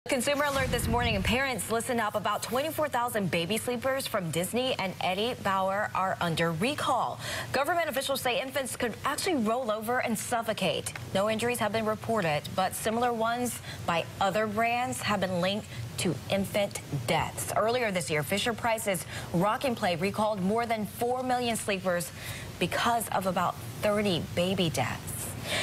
24,000 Disney and Eddie Bauer baby sleepers recalled
Infants could roll over and suffocate, officials said.